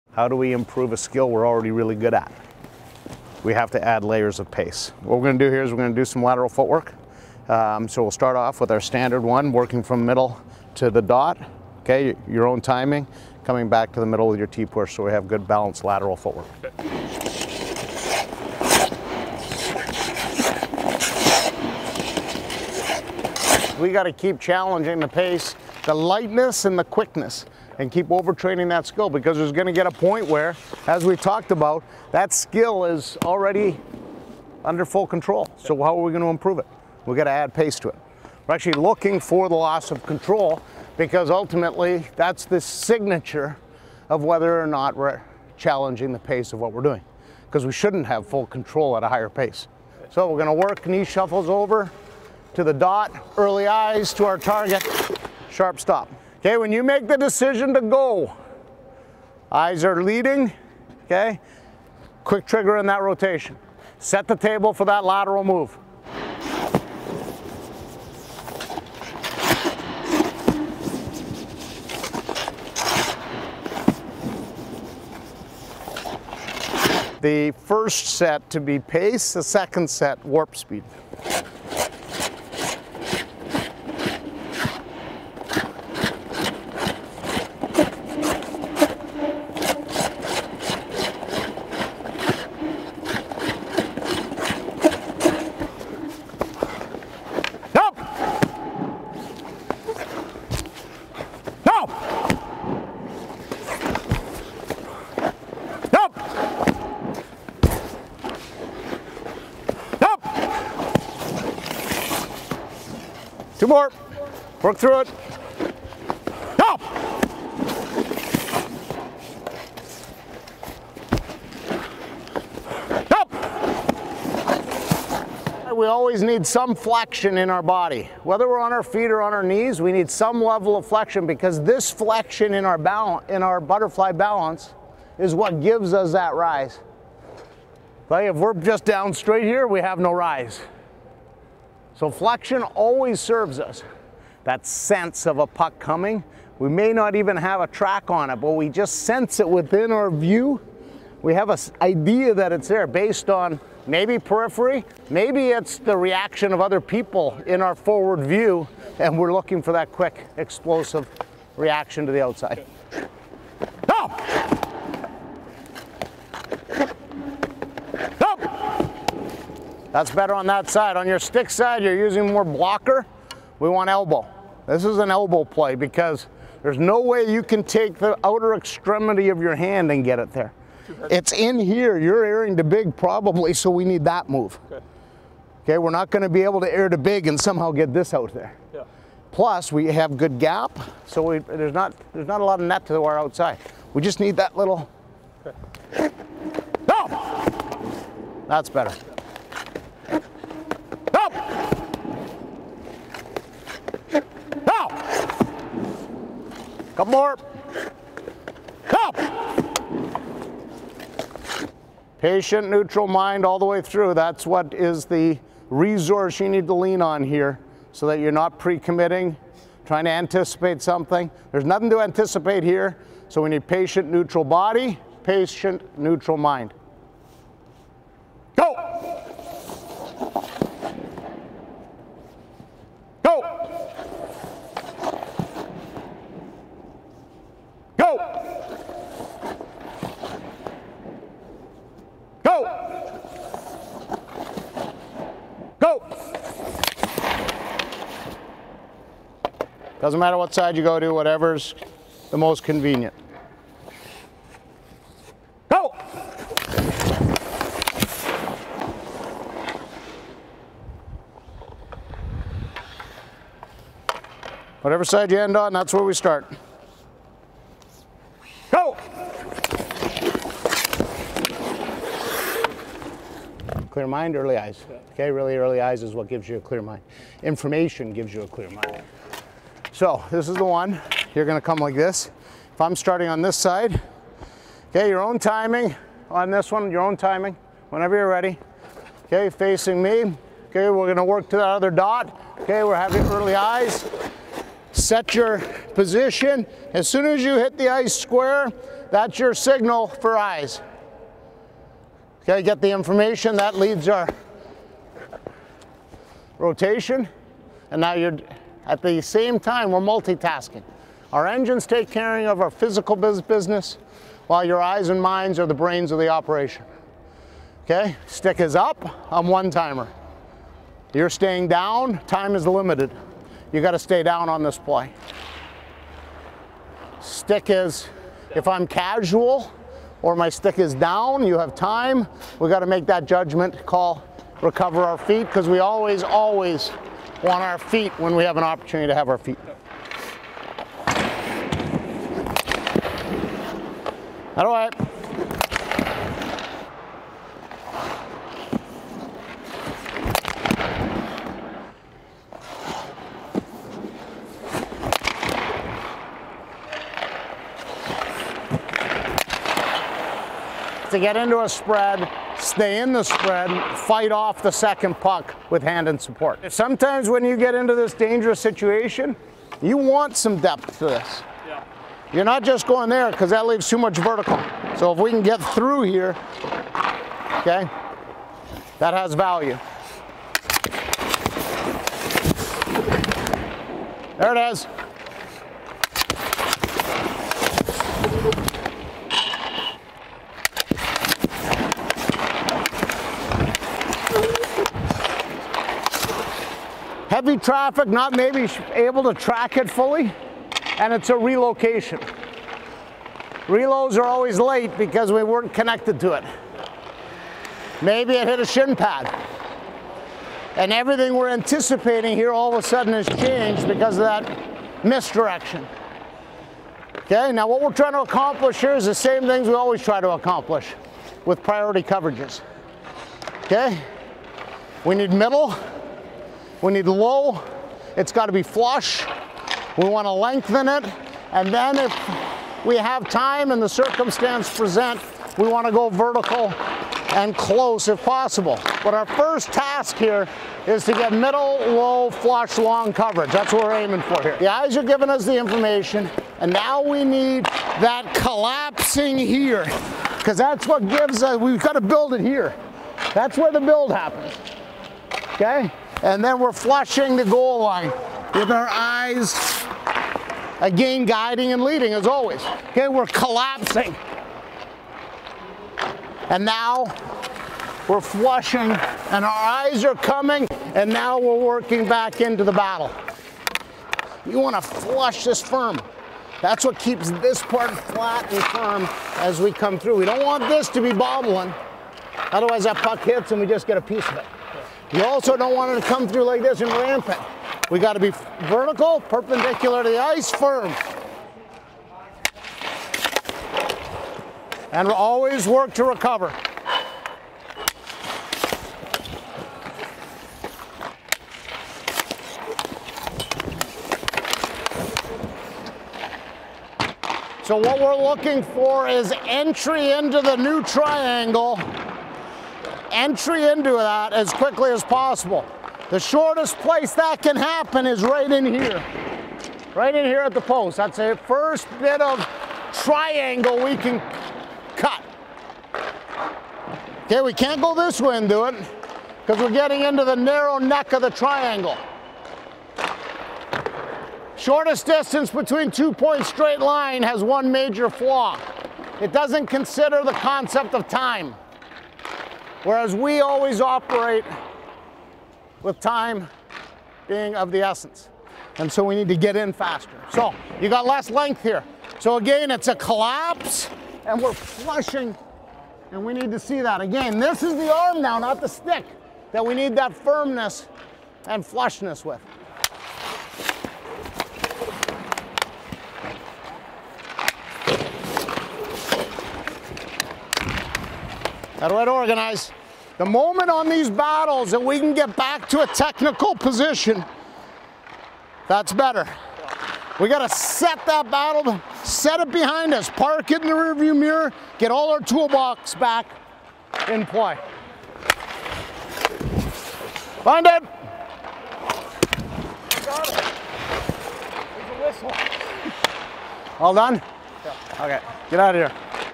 Goaltending Coach Ian Clark Mic'd Up With Thatcher Demko at Practice
"Our engines take care of our physical business, while our eyes and minds are the brains of the operation." Listen in as Canucks Goaltending Coach Ian Clark works with netminder Thatcher Demko.

Presented by Rogers.